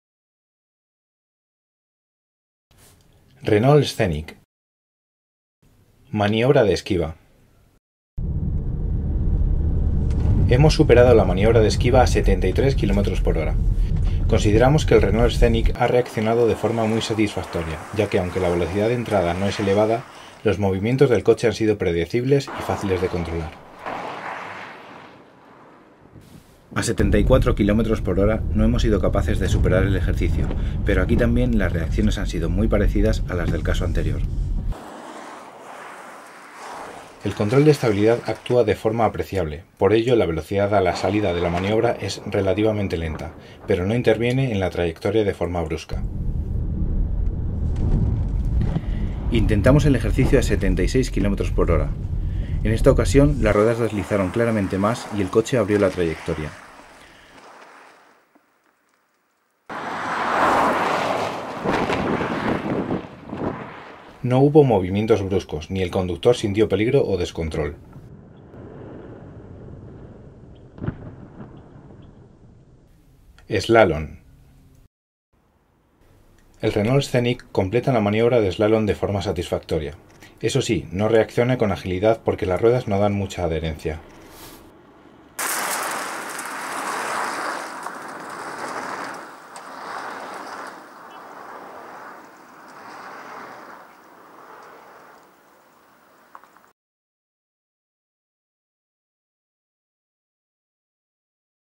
Renault Scenic Energy dCi 130 2017 - Maniobra de esquiva (moose test) y eslalon | km77.com
Prueba, imágenes, precios, seguro y fichas técnicas y de equipamiento del Renault Scenic Energy dCi 130 2017